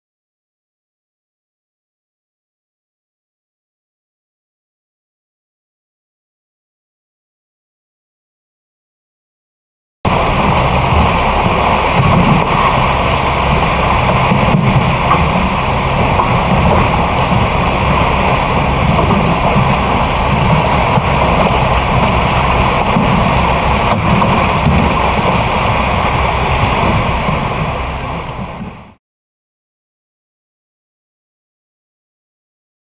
OPS releases Full Video from The Cove - Preview
The filmmakers of The Cove are releasing unedited and unaltered footage from Taiji, Japan. for the full videos from four different camera angles placed in the cove.

Read Director Louie Psihoyos's response to the article in Newsweek that prompted the release of this footage